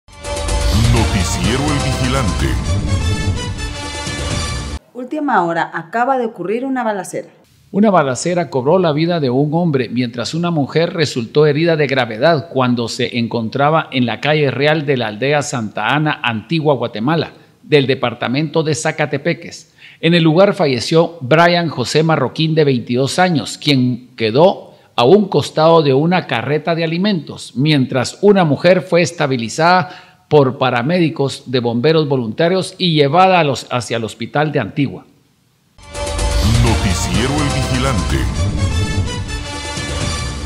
Balacera en Antigua Guatemala deja un fallecido y una mujer herida
Una balacera cobró la vida de un hombre mientras una mujer resultó herida de gravedad cuando se encontraban en la Calle Real de la aldea Santa Ana, Antigua Guatemala